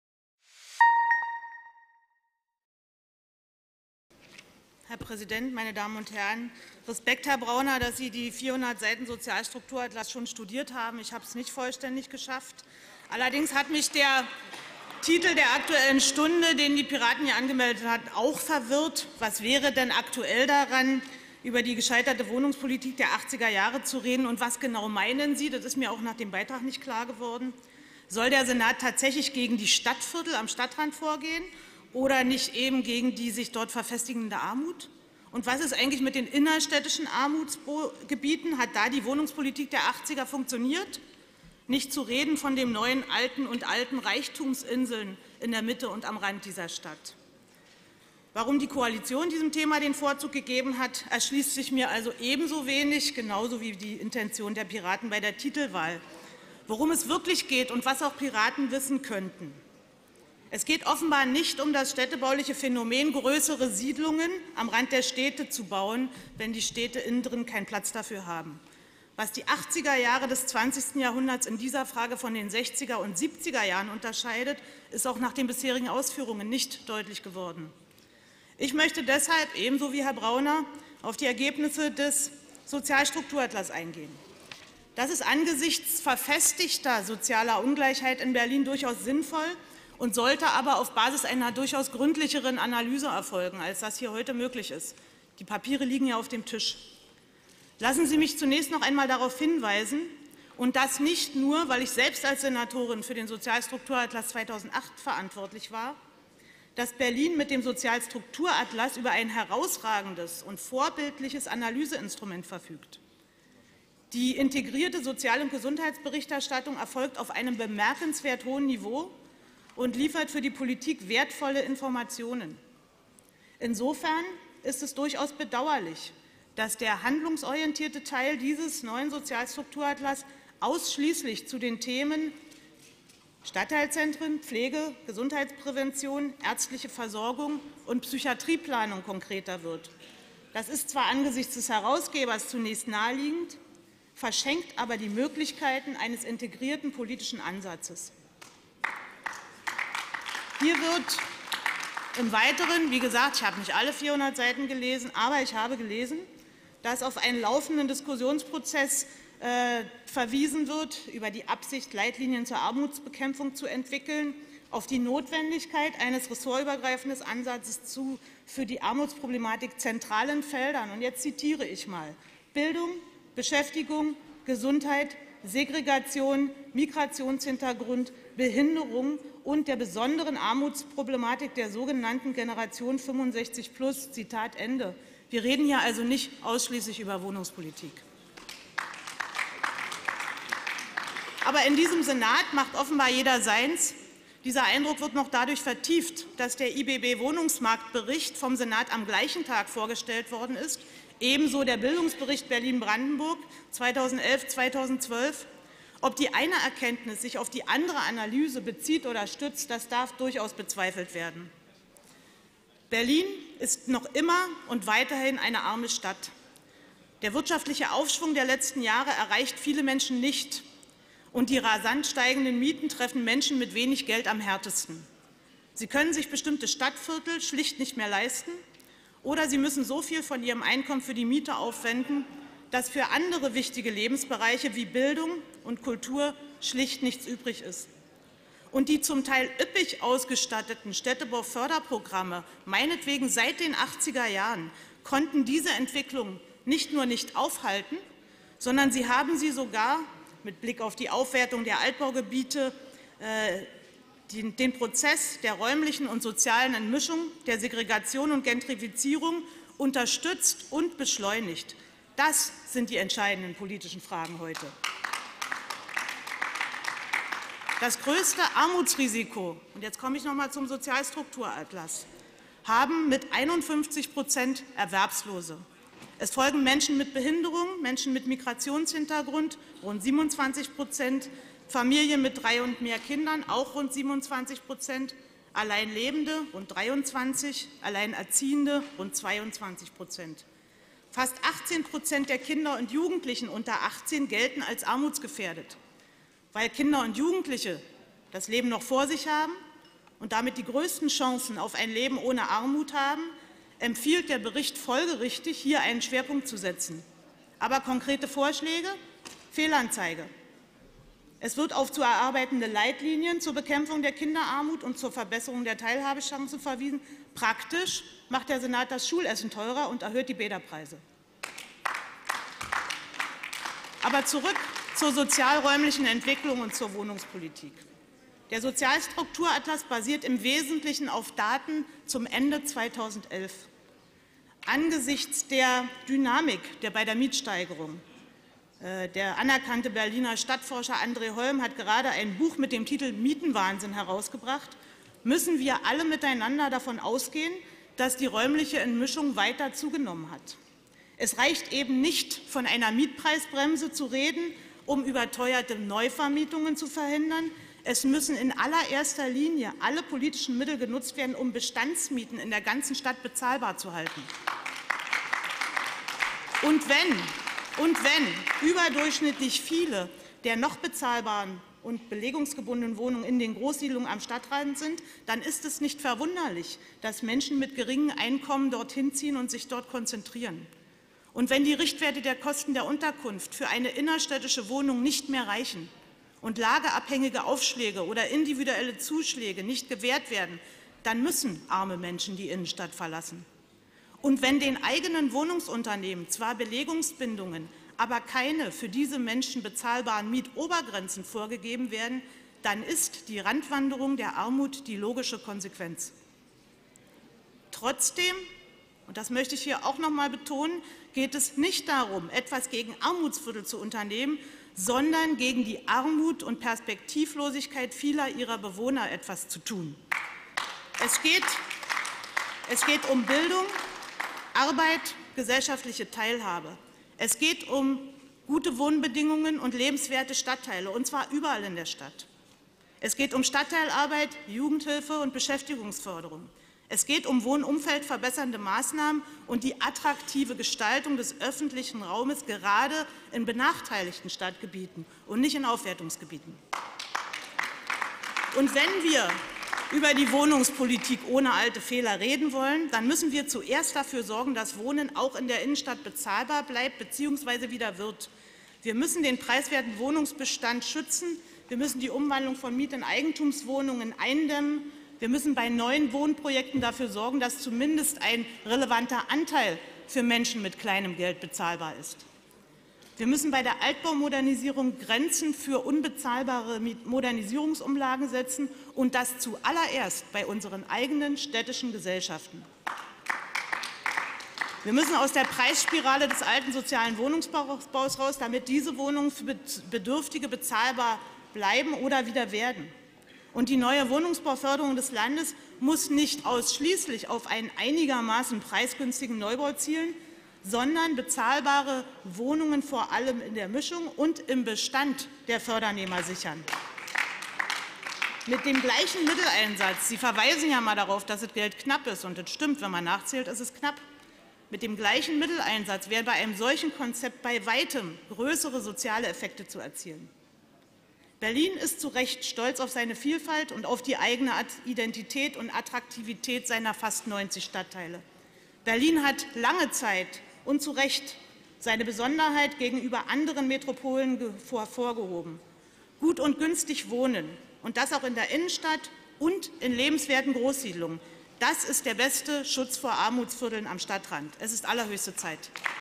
44. Sitzung - Aktuelle Stunde - Katrin Lompscher zu Gescheiterte Wohnungspolitik der 80er Jahre
44. Sitzung

fragt, was die Piraten mit ihrem Antrag wollen. Solle der Senat etwa gegen die Großsiedlungen am Stadtrand vorgehen? Es gehe nicht ausschließlich um Wohnungspolitik. Es müsse etwas gegen Armut und Perspektivlosigkeit der Bewohner getan werden. Wenn man eine neue Wohnungspolitik wolle, müsse man allerdings auch bezahlbaren Wohnraum in den Innenstädten schaffen, die Umwandlung von Miet- in Eigentumswohnungen bremsen und die Vermietung von Ferienwohnungen reduzieren. Die Menschen sollten sowohl in der Innenstadt als auch in lebenswerten Großsiedlungen gut und günstig wohnen können.